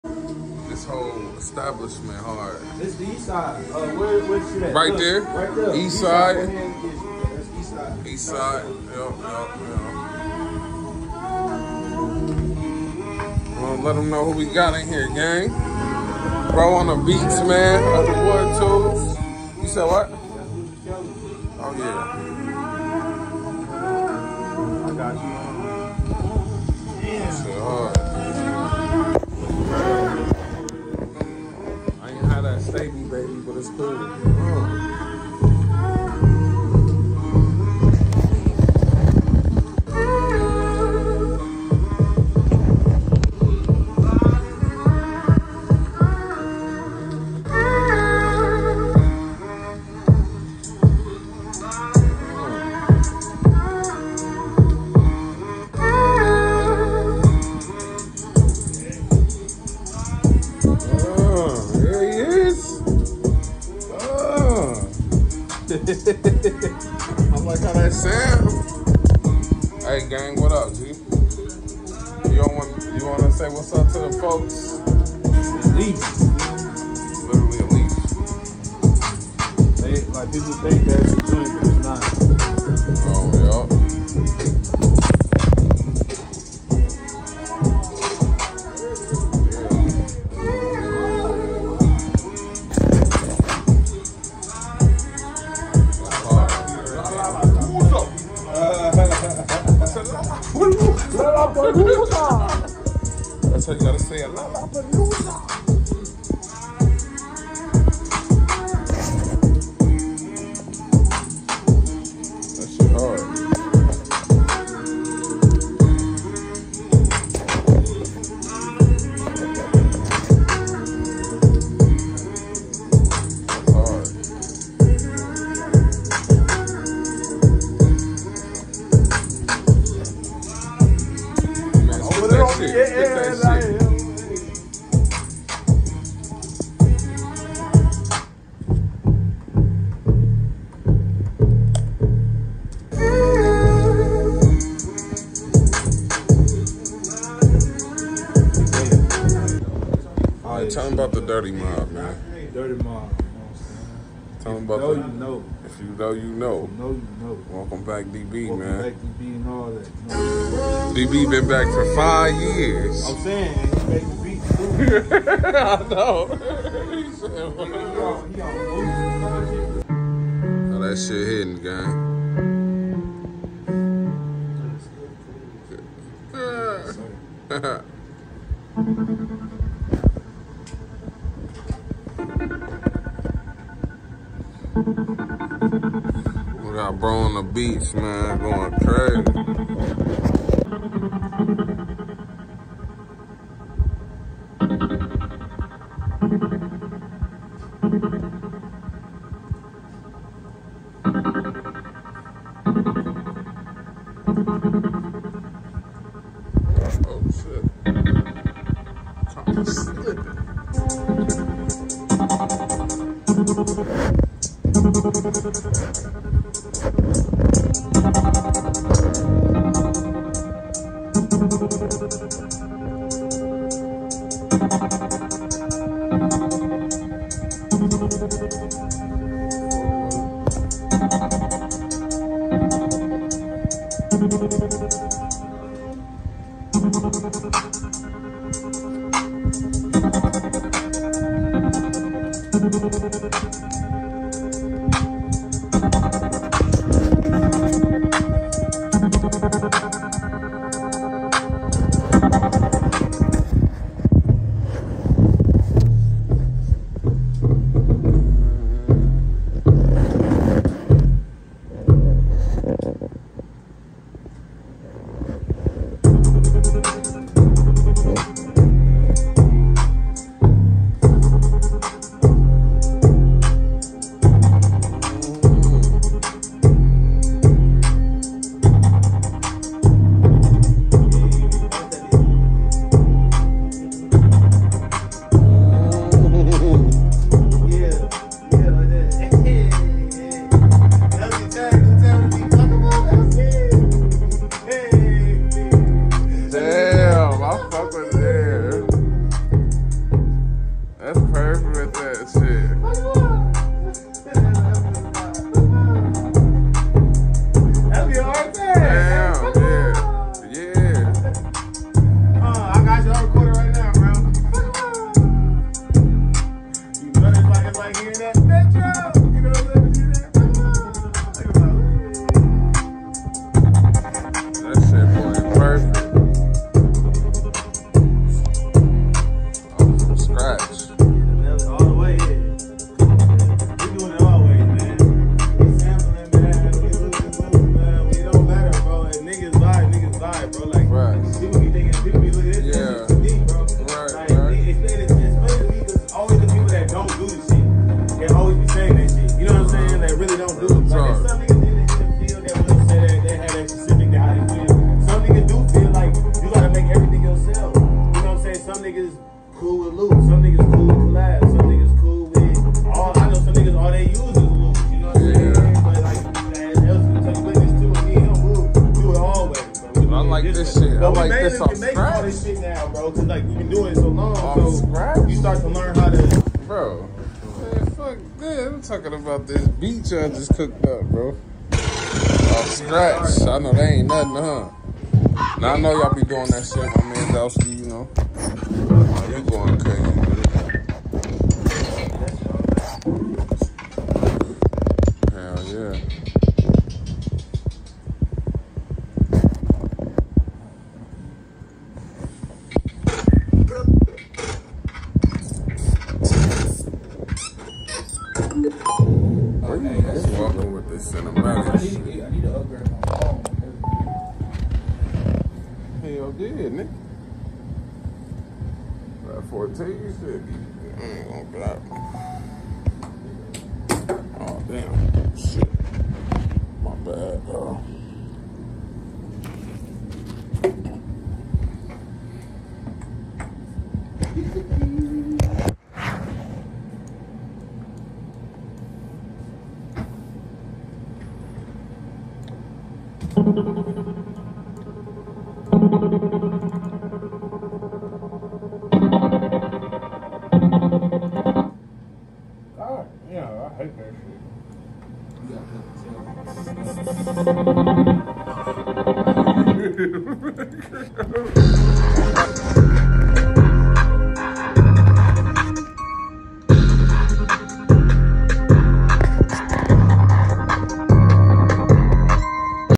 EastSide Studio Vlog | FL Studio | Lskii Wtc
Cooling cooking up beats. Playing wit new sounds. 2023 shoutout all my woes 💯